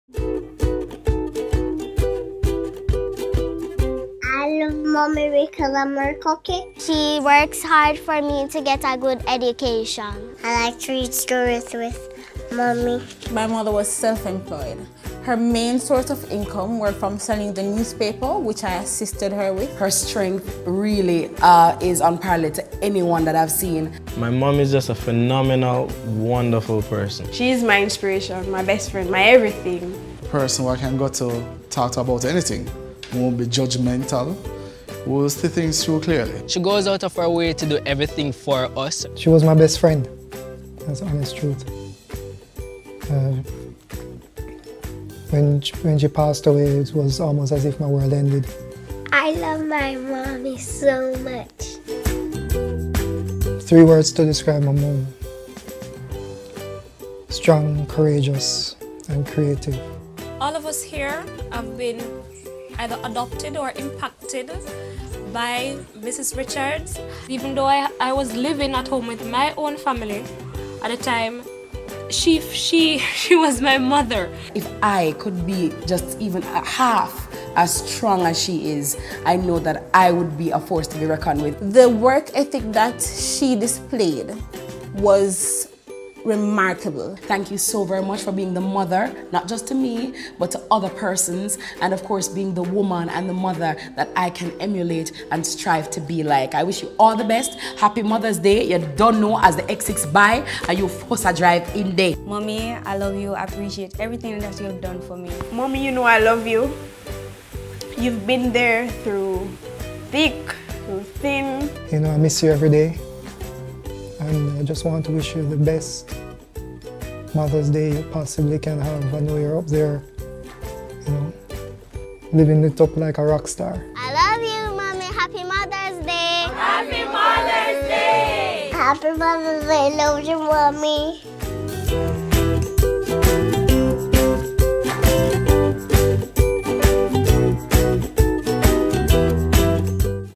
Why Do You Love Your Mother?
A few Jamaicans telling us exactly why they love their moms.